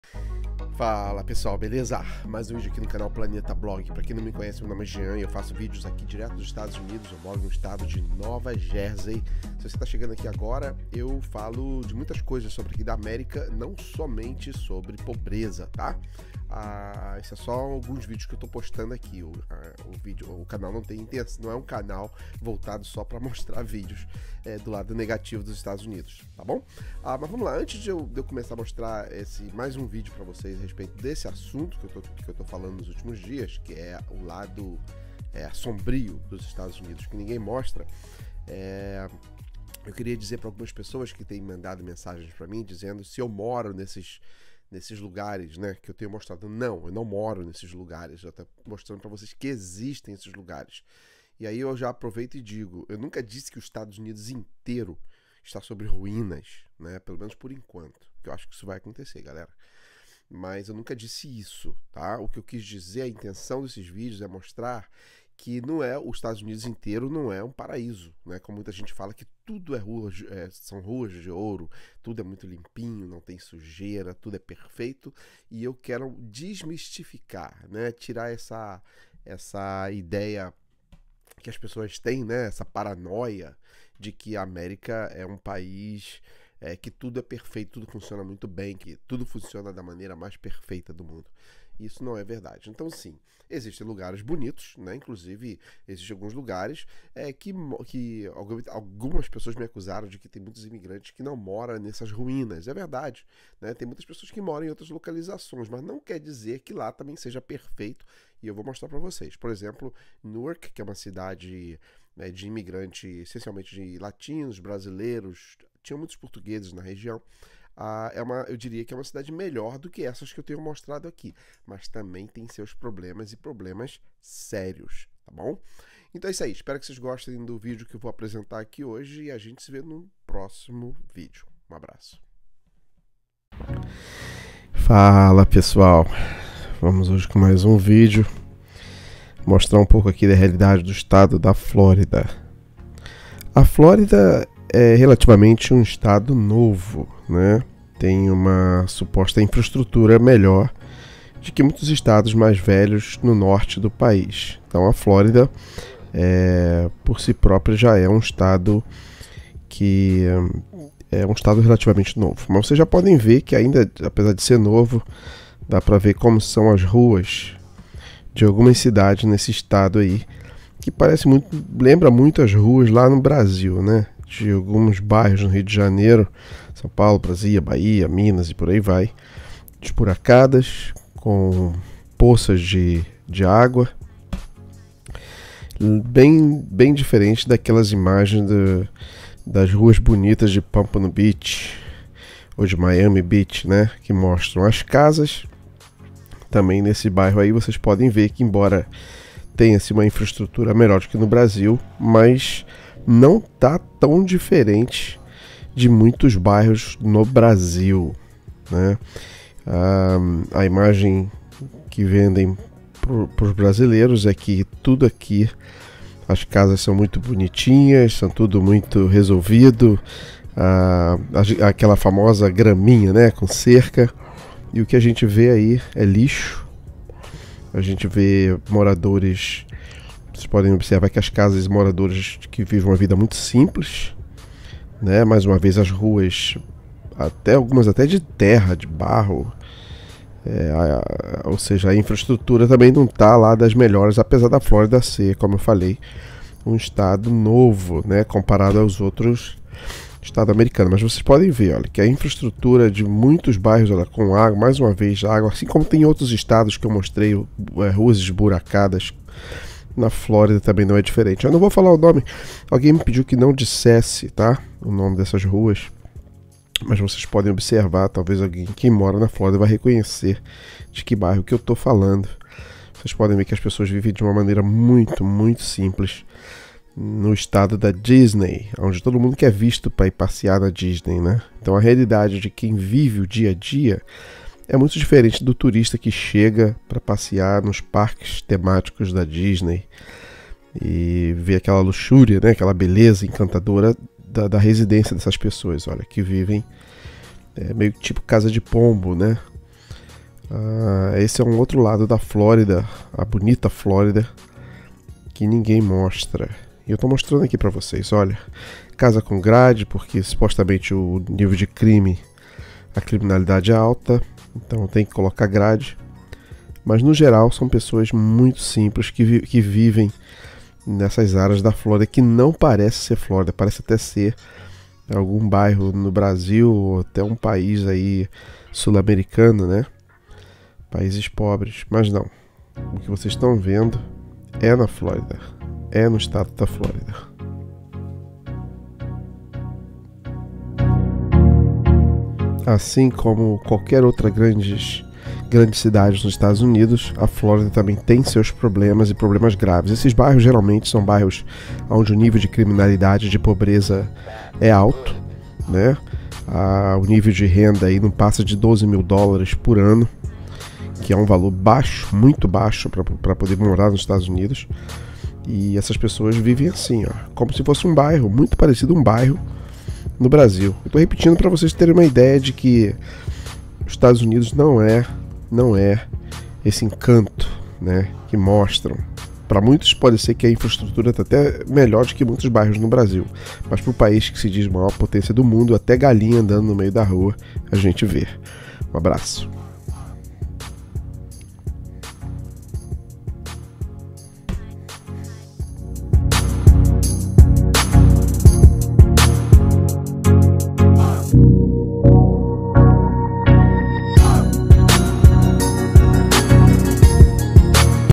Estados Unidos: pobreza FLÓRIDA que ninguém mostra
pobreza e vida simples na Flórida

credits:
the best youtuber ever.
CharlieBo313